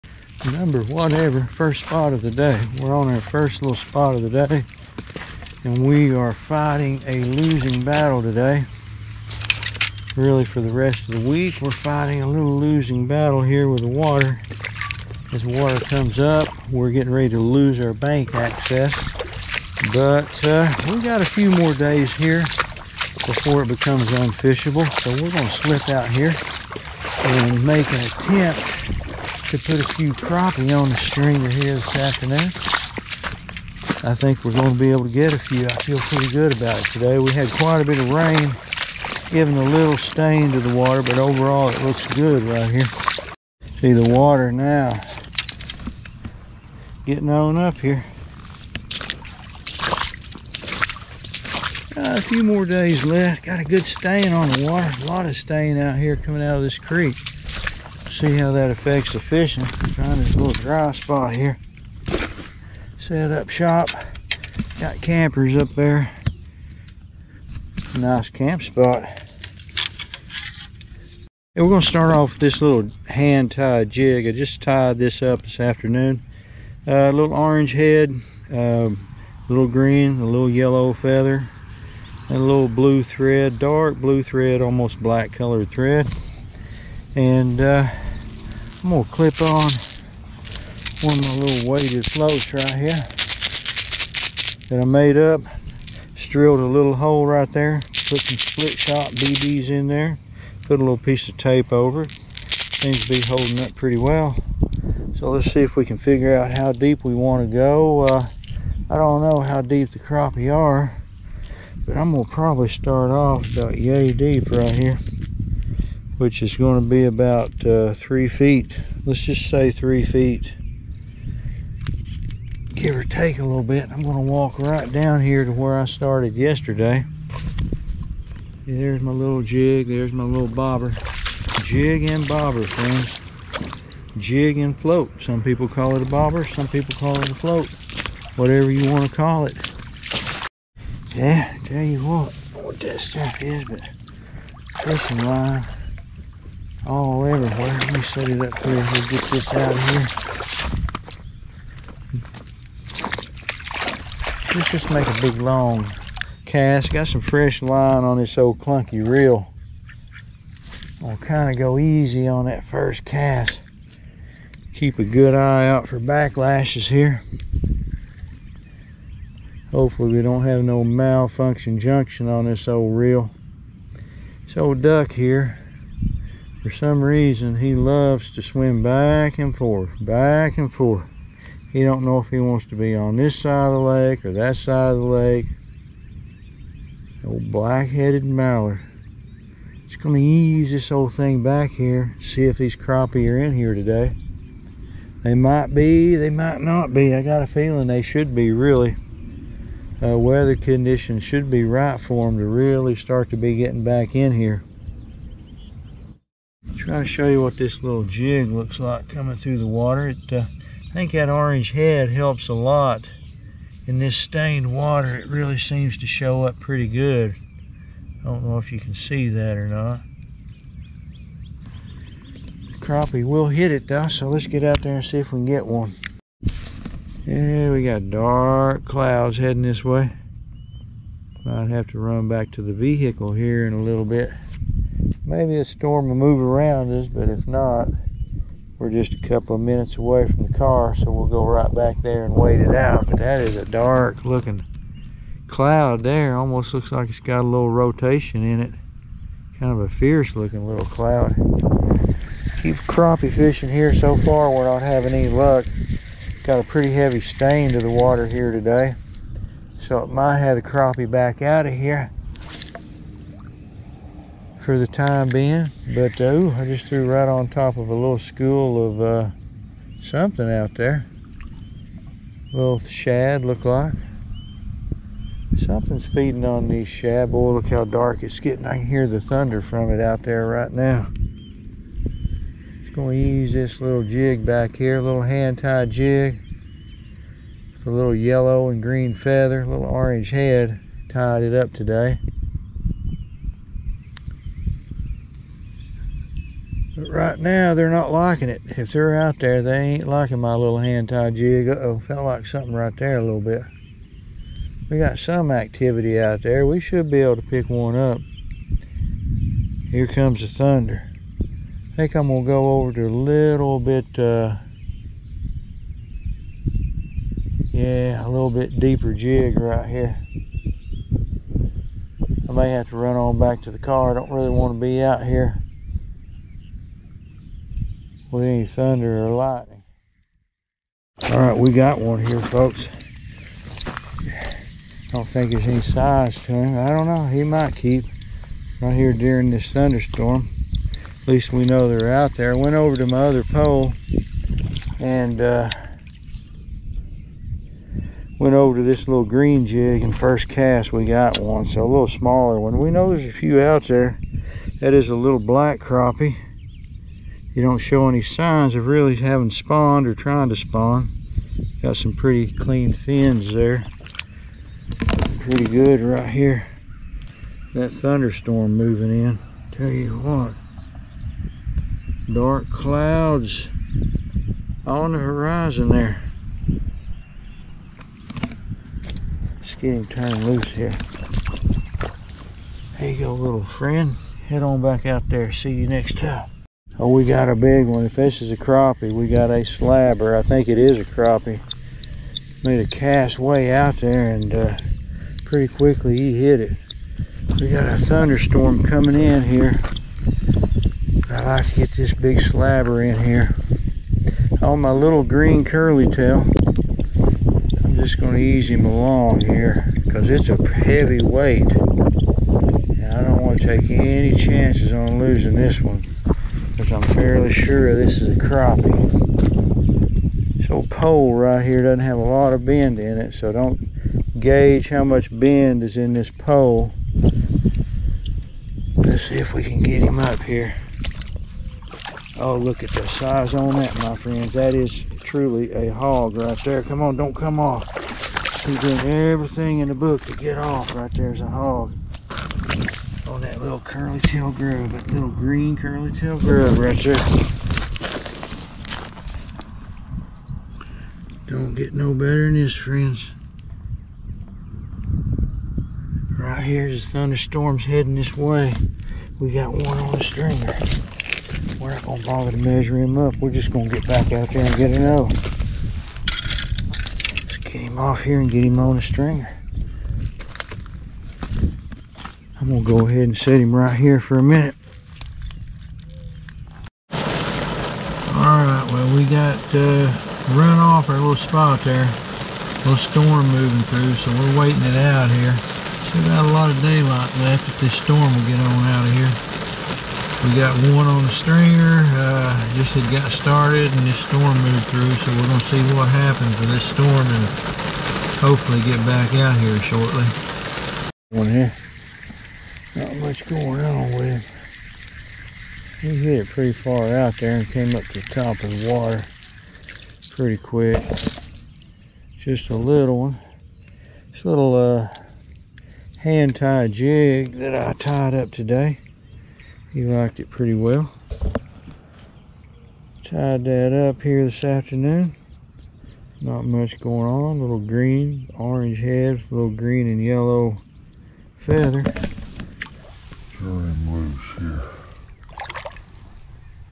Crappie Bank Fishing Jig and Bobber (Spring Spawn Underway)
In this crappie fishing video, we are walking the bank looking for some early spawn crappie here in East Tennessee. Fishing with small jigs under a bobber or float